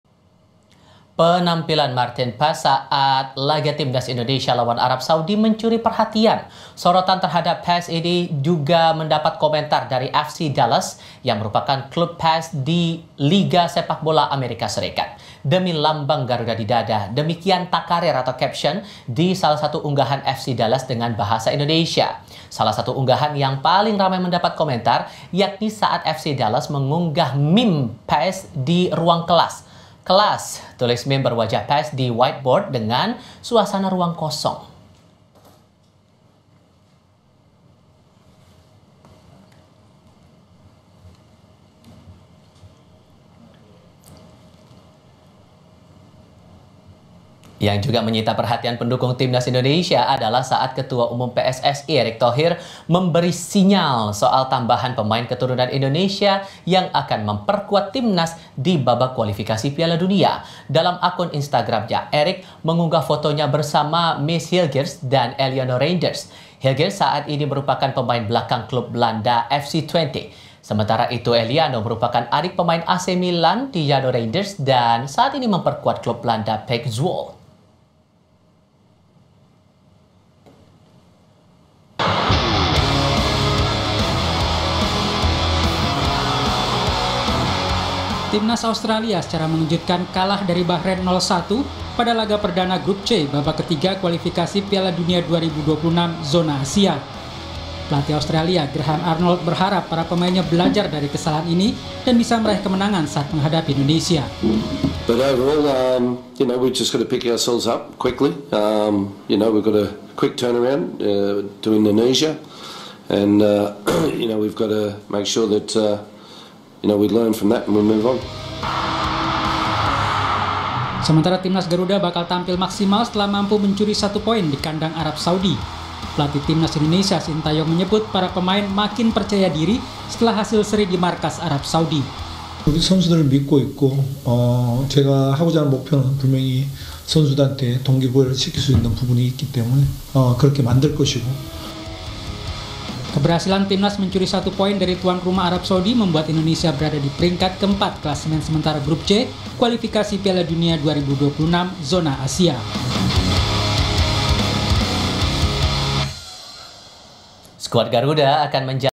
Unggahan Lucu FC Dallas Komentari Aksi Maarten Paes & Dua Pemain Baru Timnas
KOMPAS.TV-Unggahan Lucu FC Dallas Komentari Aksi Maarten Paes & Dua Pemain Baru Timnas